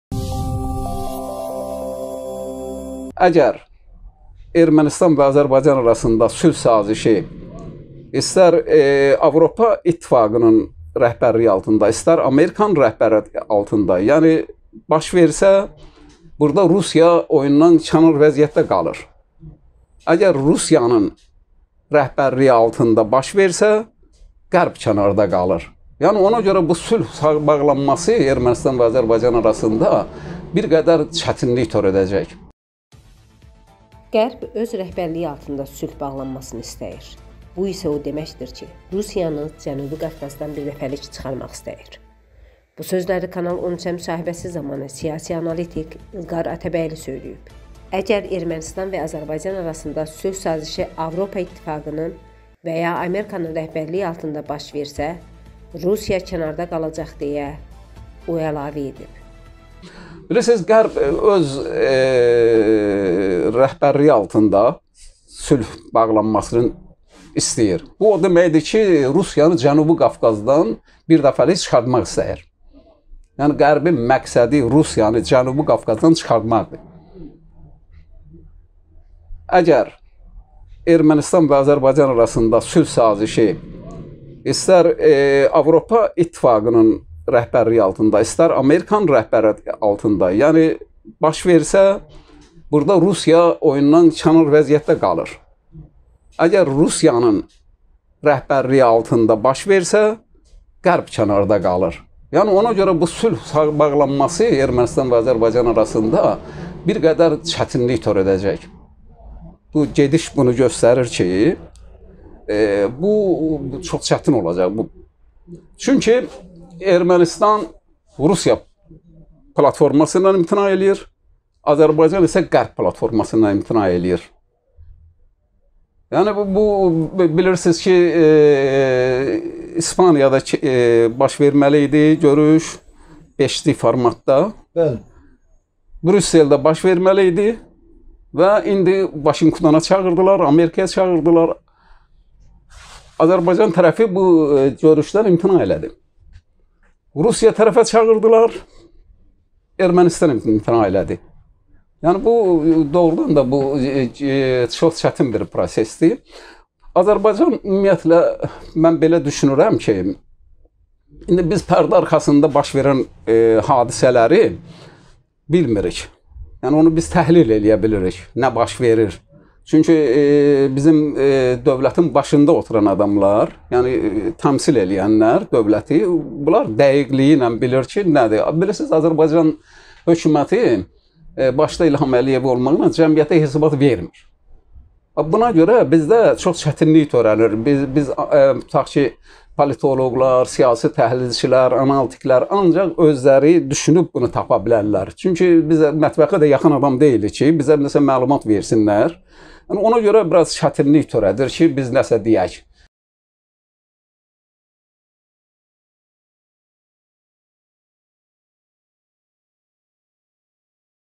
"Qərbin məqsədi Rusiyanı Cənubi Qafqazdan ÇIXARMAQDIR" - İlqar Atabəyli
"Qərb öz rəhbərliyi altında sülh bağlanmasını istəyir. Bu isə o deməkdir ki, Rusiyanı Cənubi Qafqazdan birdəfəlik çıxarmaq istəyir"
Bu sözləri Kanal13-ə müsahibəsi zamanı siyasi analitiq İlqar Atabəyli söyləyib.
"Əgər Ermənistan və Azərbaycan arasında sülh sazişi Avropa ittifaqının və ya Amerikanın rəhbərliyi altında baş versə, Rusiya kənarda qalacaq"-deyə, o əlavə edib.